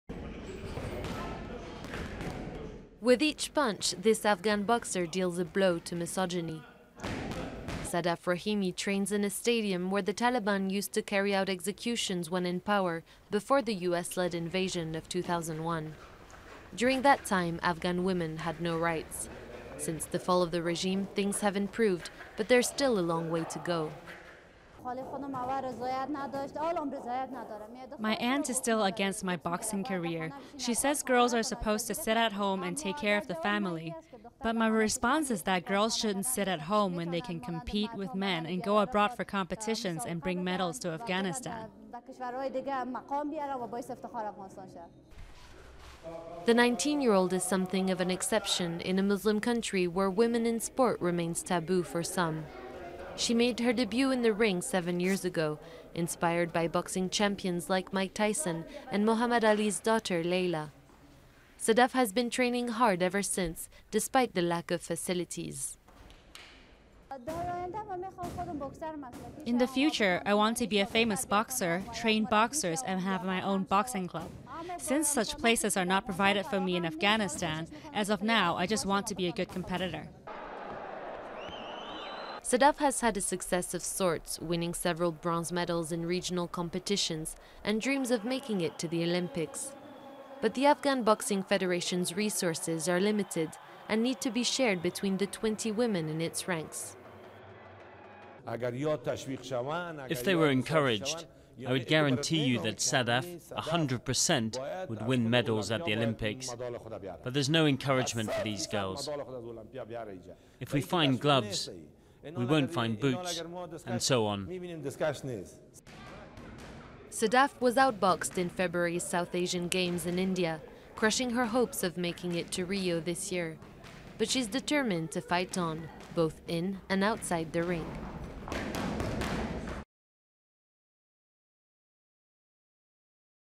Young Afghan boxer packs a punch against patriarchy
Nineteen-year-old Afghan boxer Sadaf Rahimi trains in the bowels of Kabul's Ghazi stadium, dealing blows to gender stereotyping and doing her part to to exorcise history: it was on these very grounds that the Taliban carried out public executions.